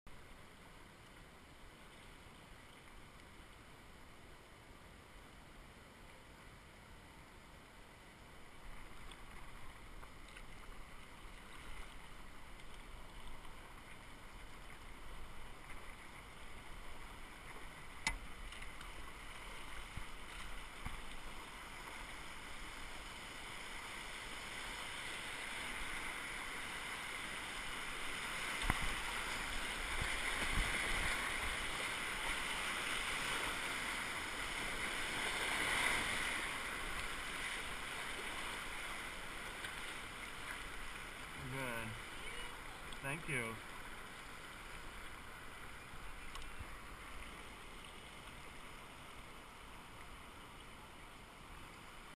Ledge Rapid
Helmetcam of running Ledge Rapid, San Juan River, 08/2012